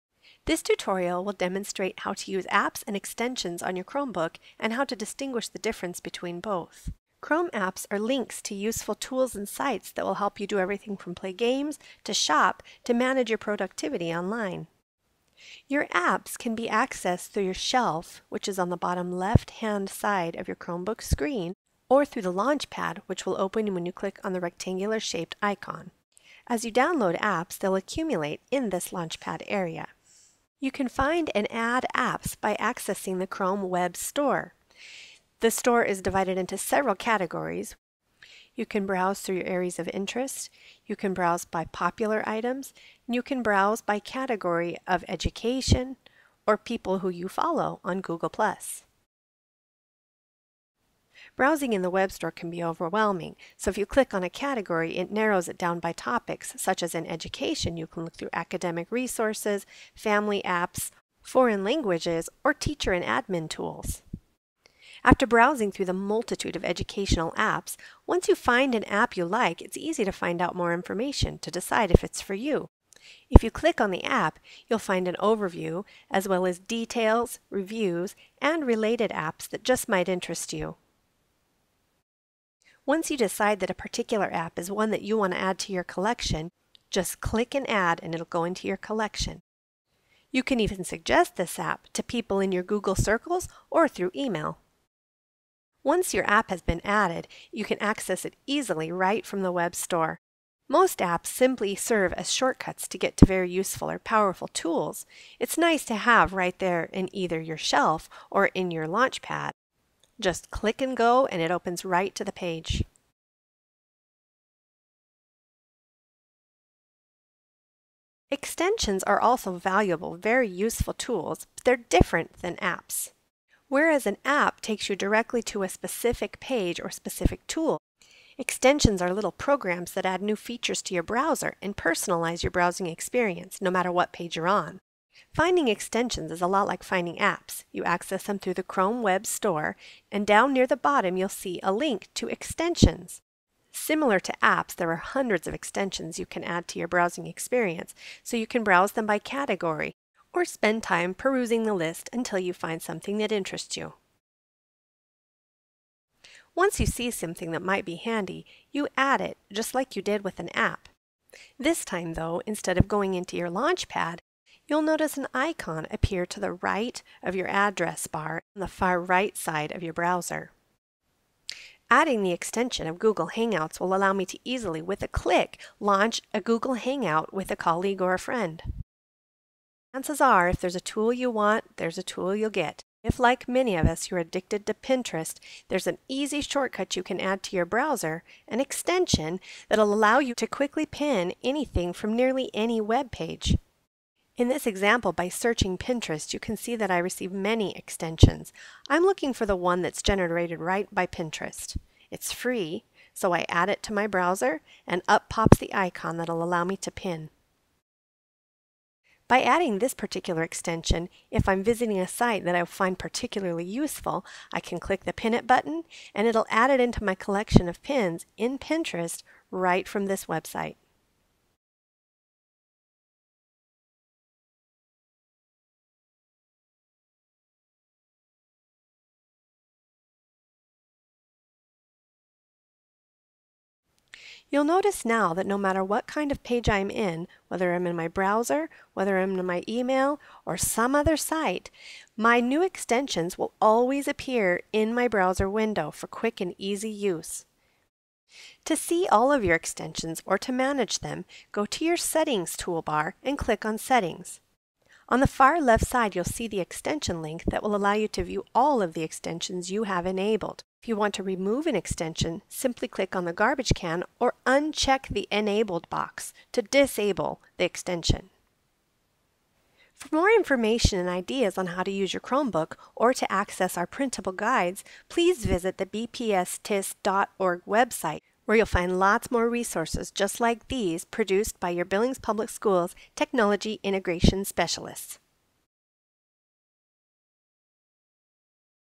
Chromebook Apps and Extensions
Learn to add and manage apps and extensions in Google Chrome.